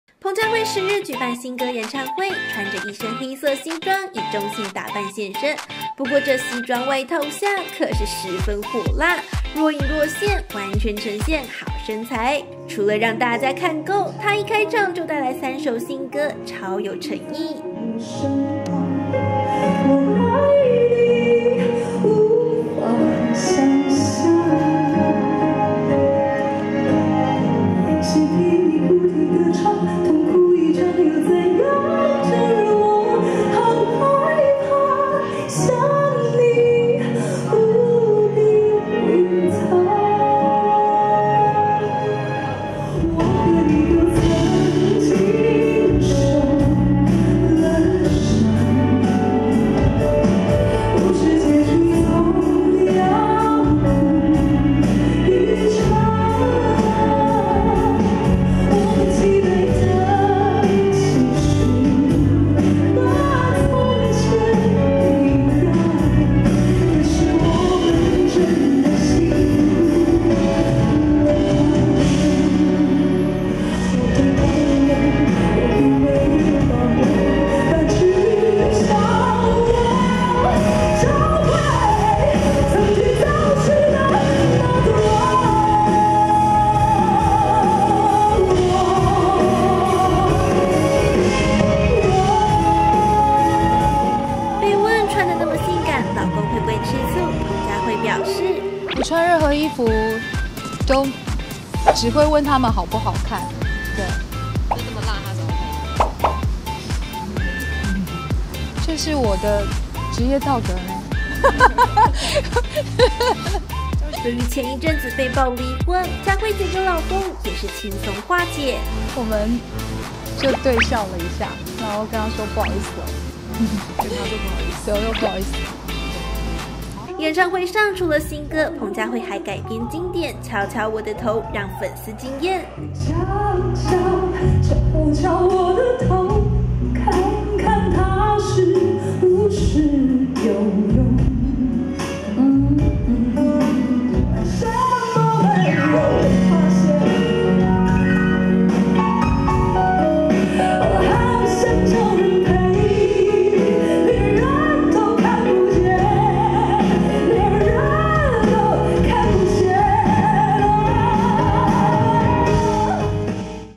【2年前】彭佳慧性感開唱 跟老公聊婚變過程超尷尬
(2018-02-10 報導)

彭佳慧10日舉辦新歌演唱會
穿著一身黑色西裝
以中性打扮現身
不過這西裝外套下可是十分火辣
若影若現
完全呈現好身材